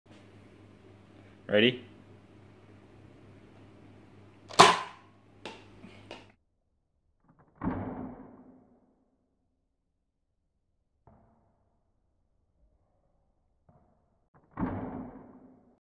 Nerf Gun Boob Shot
What does a nerf gun shot to the boob look like?